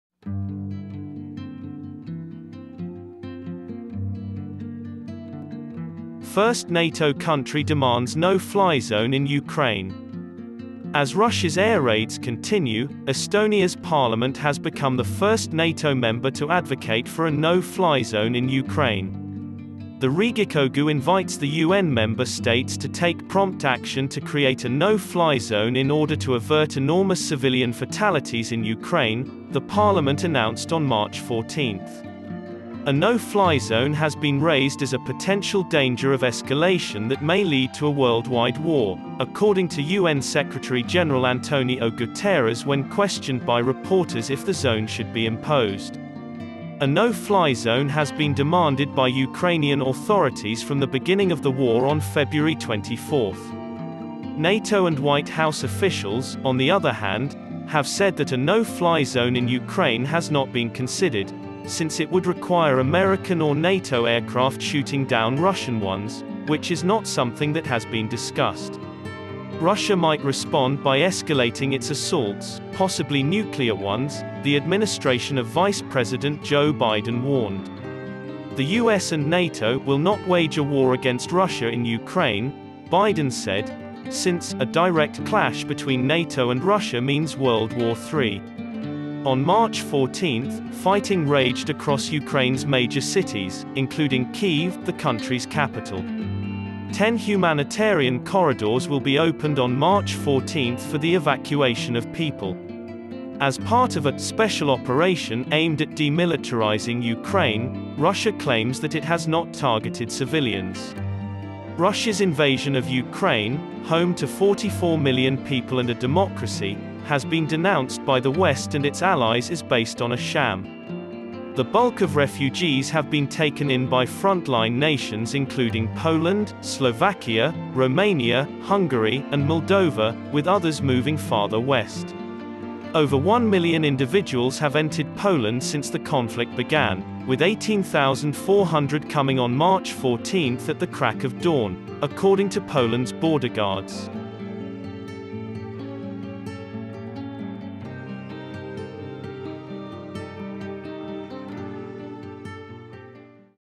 First NATO Country Demands No-Fly Zone in Ukraine | Ukraine News | NewsRme
As Russia's air raids continue, Estonia's parliament has become the first NATO member to advocate for a no-fly zone in Ukraine.
“The Riigikogu invites the UN member states to take prompt action to create a no-fly zone in order to avert enormous civilian fatalities in Ukraine,” the Parliament announced on March 14.

A no-fly zone has been raised as a potential danger of escalation that may lead to a worldwide war, according to UN Secretary-General António Guterres when questioned by reporters if the zone should be imposed.

A no-fly zone has been demanded by Ukrainian authorities from the beginning of the war on February 24. NATO and White House officials, on the other hand, have said that a no-fly zone in Ukraine has not been considered, since it would require American or NATO aircraft shooting down Russian ones, which is not something that has been discussed.